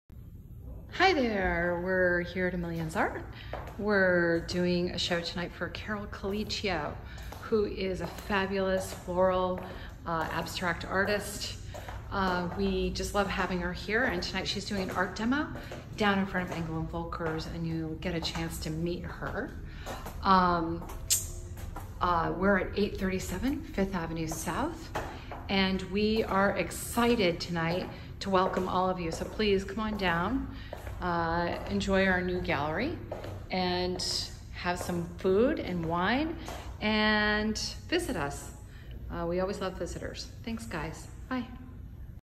Carol Calicchio Art Demo & Show
Here's a tour of abstract artist Carol Calicchio's upcoming show!